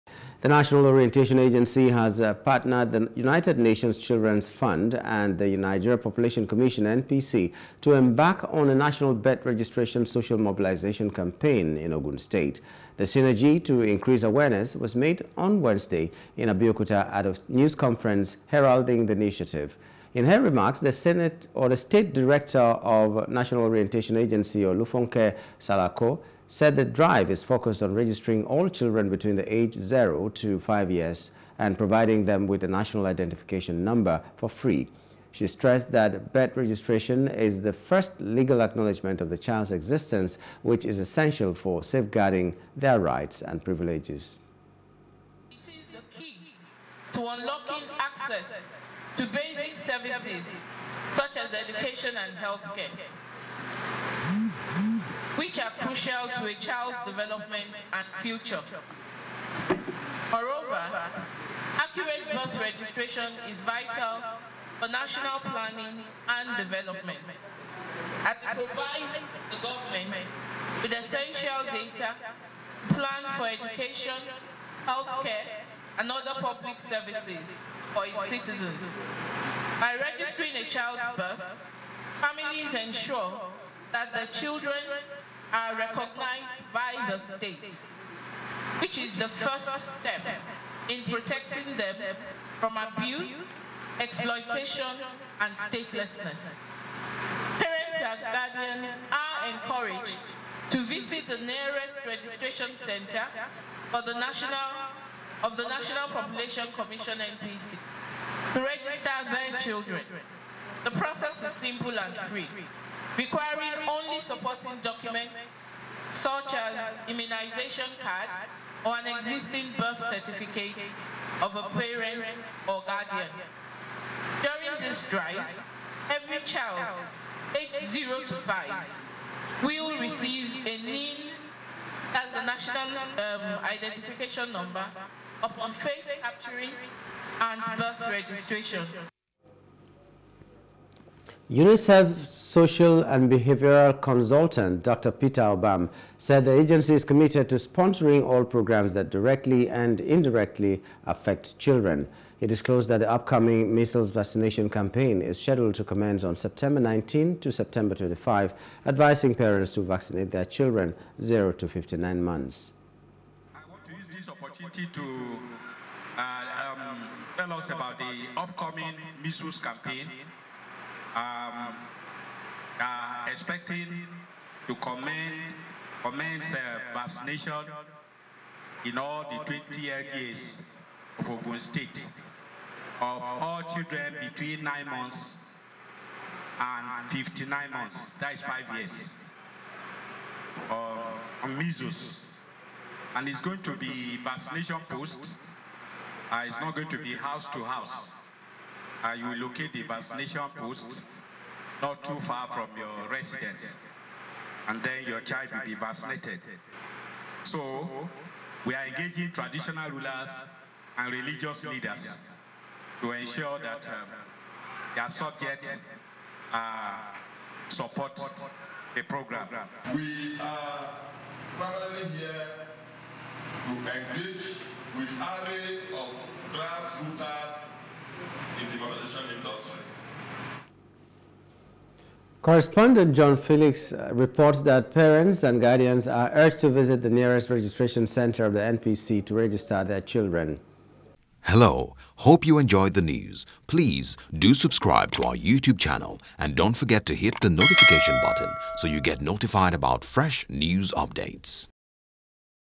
E- Birth: N.O.A. Partners U.N.I.C.E.F., NPC To Increase E- Birth Registration
The National Orientation Agency has partnered with the United Nations Children’s Fund and the Nigeria Population Commission (NPC) to embark on a national birth registration social mobilization campaign in Ogun state.
The synergy to increase awareness was made on Wednesday in Abeokuta at a news conference heralding the initiative.
In her remarks, the State Director of NOA, Olufunke Salako said the drive is focused on registering all children between the age of zero to five years and providing them with a National Identification Number for free.
She stressed that birth registration is the first legal acknowledgement of a child’s existence which is essential for safeguarding their rights and privileges.